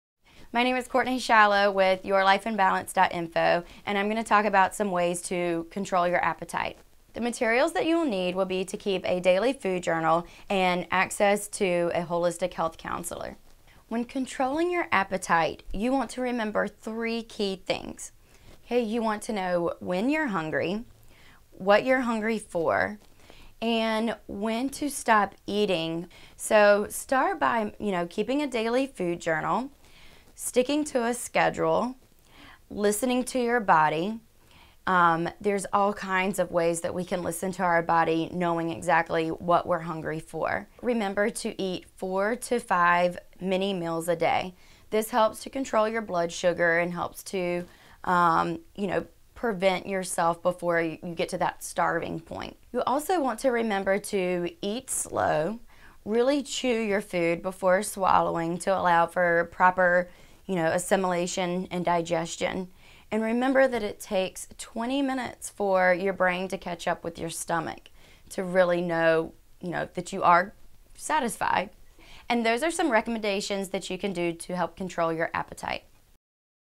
Healthy Eating : How to Control Your Appetite
Gaining control of one's appetite means knowing when there is hunger, what the body is hungry for and when to stop eating. Learn how to use a food journal to control overeating with help from a holistic health counselor in this free video on healthy eating and appetite.

Bio: Cortney Shallow is a certified Holistic Health Counselor (HHC) at the Natural Therapies Institute in Wilmington, North Carolina.

Series Description: